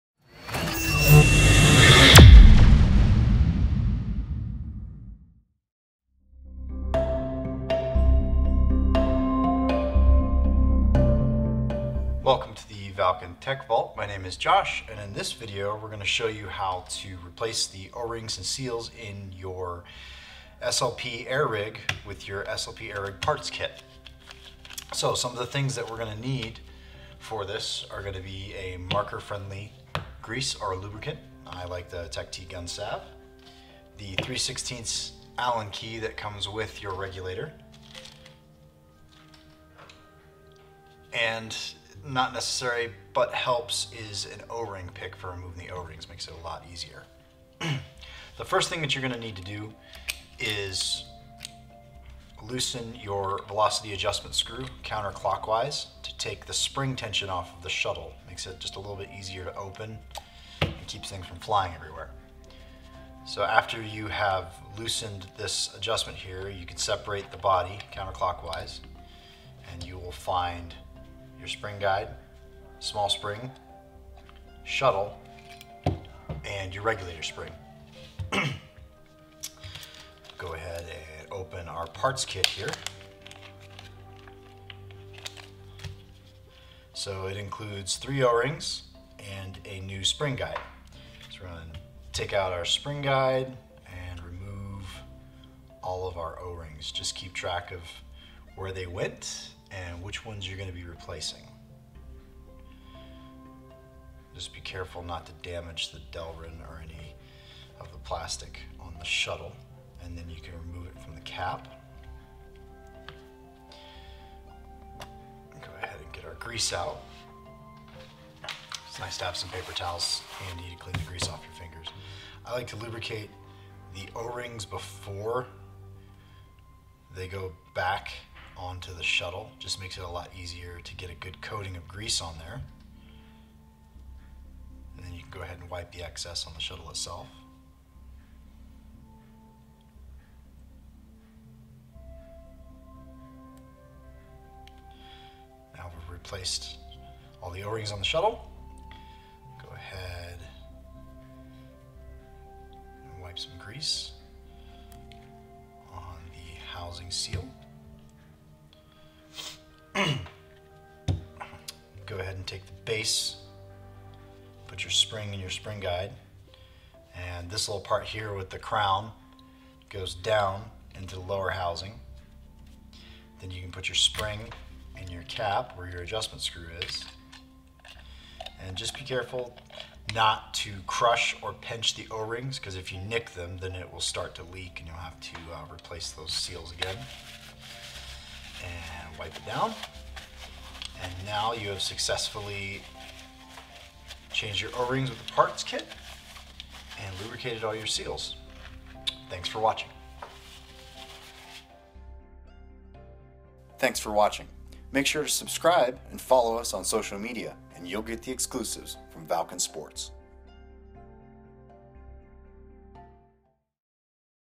Valken Tactical Tech Vault SLP QD Air Rig Rebuild Kit
In this Tech Vault installment Josh shows you how to properly install and lubricate the Rebuild Kit for the Valken SLP QD Air Rig system.